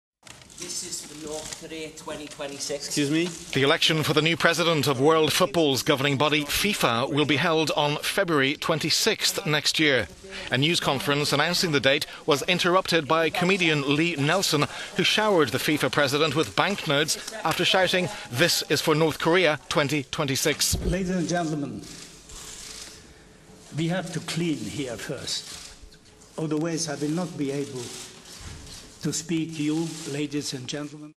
FIFA: presidential election set for February 26, 2016
FIFA has set a date to elect a new president.

An extraordinary congress will be held at the organisation's headquarters in Zurich in Switzerland on the 26th of February 2016.